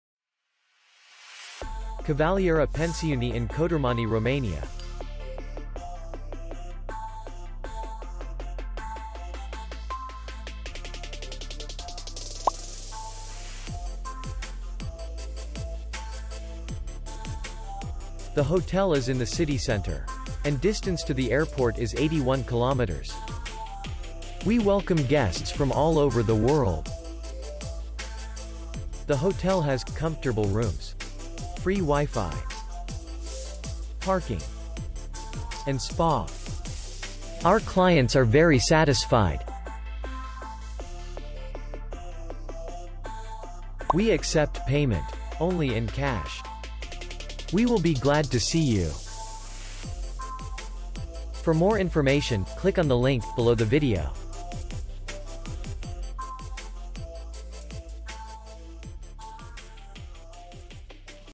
Kavalliera Pensiune hotel review | Hotels in Cotormani | Romanian Hotels
Kavalliera Pensiune hotel video review. Hotels in Cotormani, european 3 stars hotels.